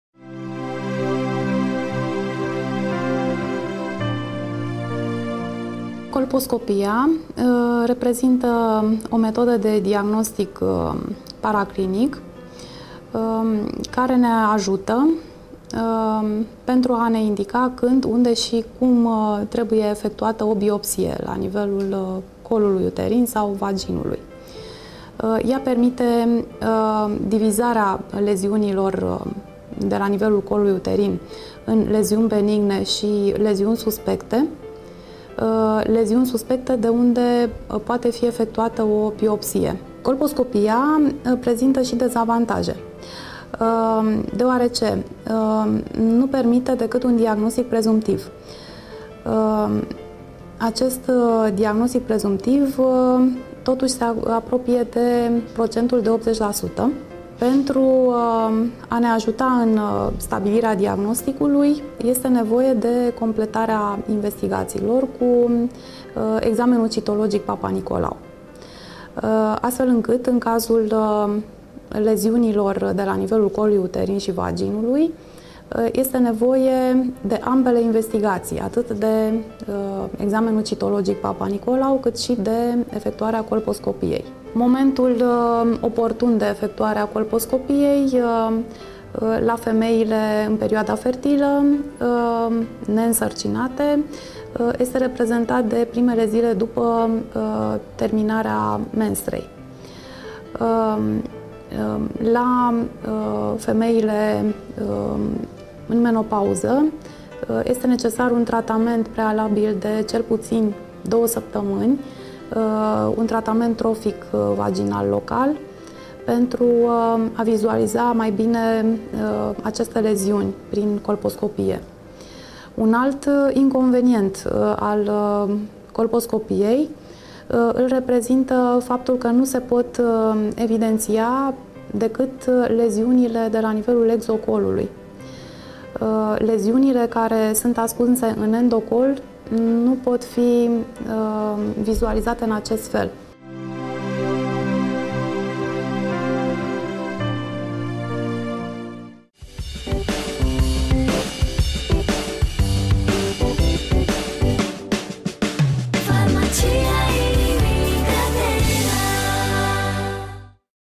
Importanţa colposcopiei
Dr.Irina Stelea - medic primar obstetrică şi ginecologie, competenţă ecografie - vorbeşte despre colposcopie.Colposcopia este indicată în cazul în care Testul Papanicolau indică prezenţa unor leziuni ce necesită investigaţii suplimentare sau rezultatele testului PAP sunt incerte
SENSO TV